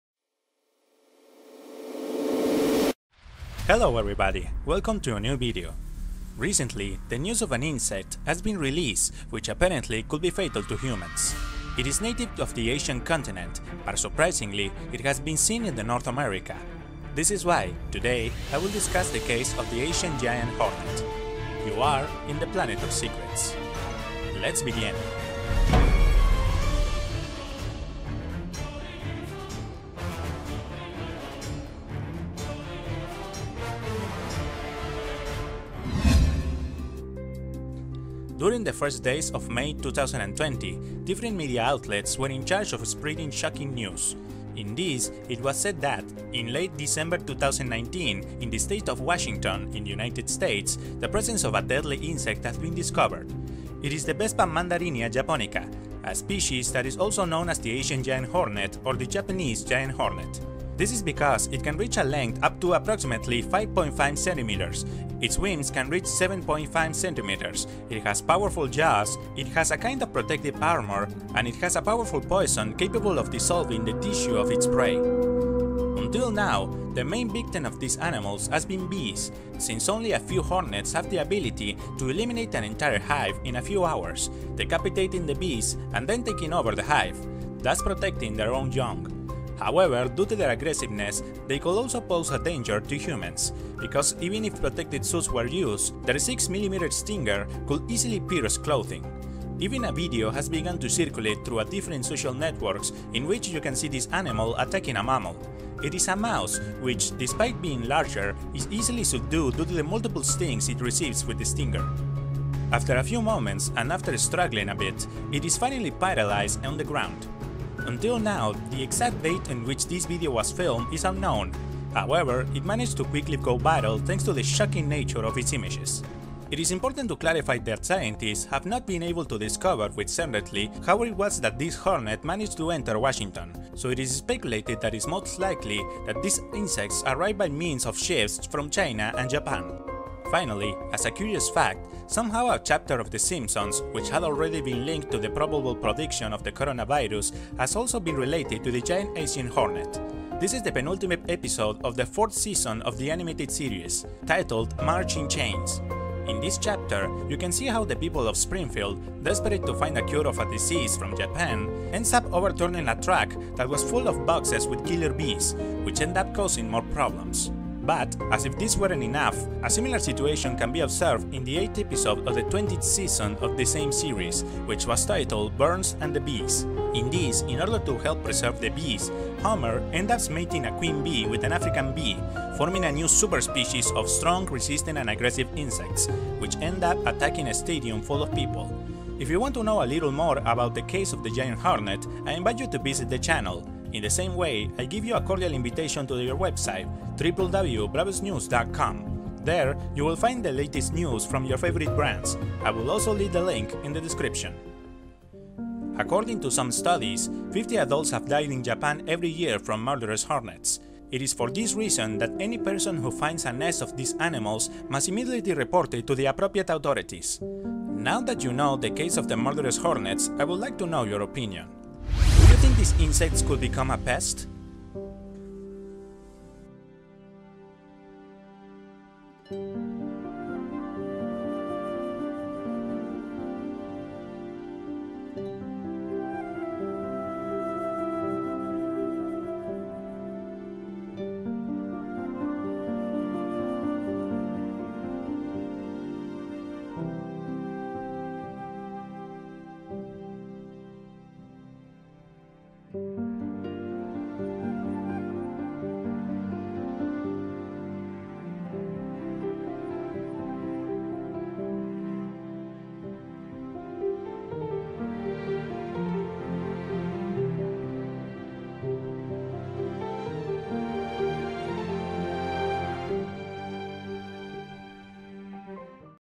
The DARK SECRETS Of MURDER HORNET
Recently the news of an insect has been released which apparently could be fatal to humans.
It is native to the Asian continent, but surprisingly it has been seen in North America.
This is why today I will discuss the case of the Asian giant hornet.
You are on the planet of secrets.

Audiovisual content is propriety by IrvinBravo